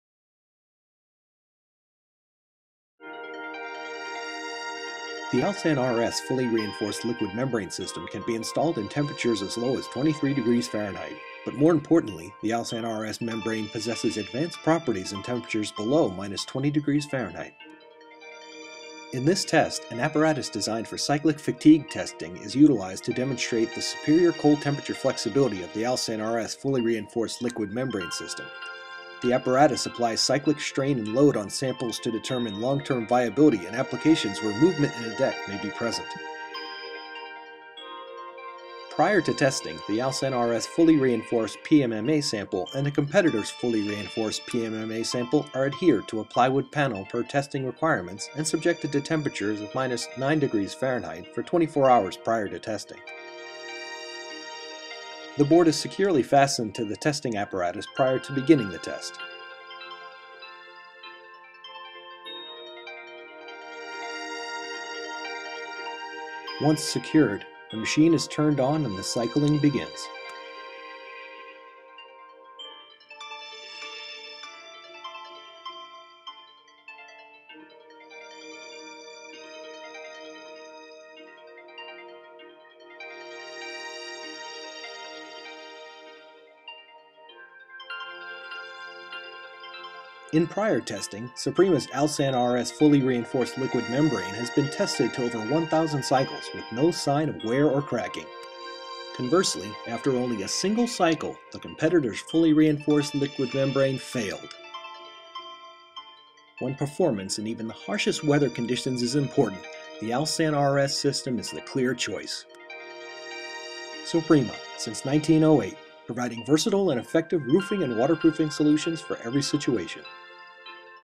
Alsan RS Cyclic Fatigue Test
Alsan RS Cyclic Fatigue test demonstrates why Alsan RS PMMA is superior to the competition.